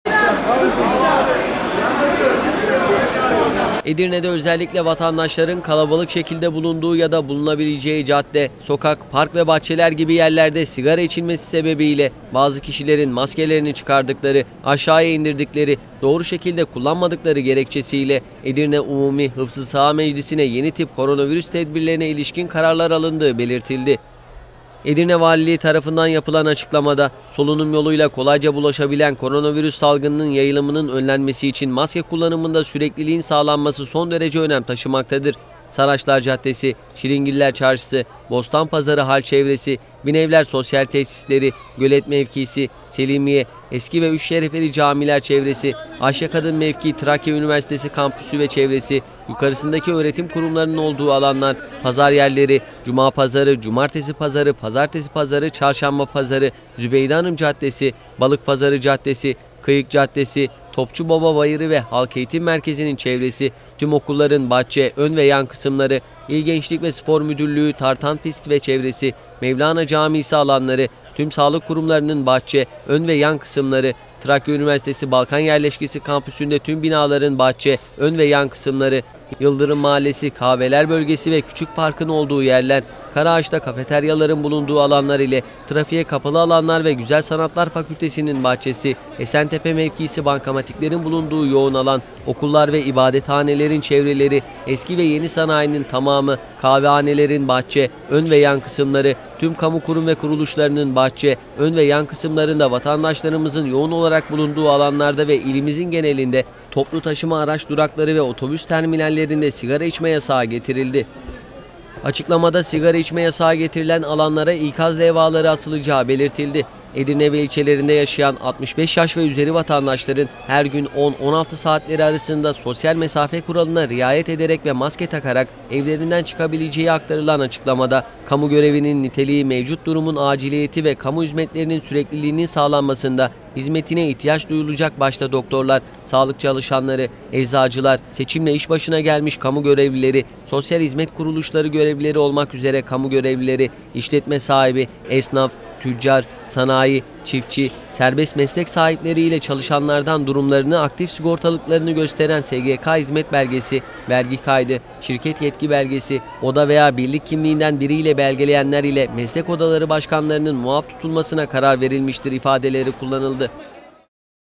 SİGARA YASAĞININ UYGULANACAĞI ALANLAR BELİRLENDİ
Edirne Valiliği, İçişleri Bakanlığının genelgesi doğrultusunda kamuya açık alanlarda sigara içme yasağının getirildiğini bildirdi. Valilikten yapılan açıklamada, Edirne Umumi Hıfzıssıhha Meclisi'nce yeni tip koronavirüs (Kovid-19) tedbirlerine ilişkin kararlar alındığı belirtildi.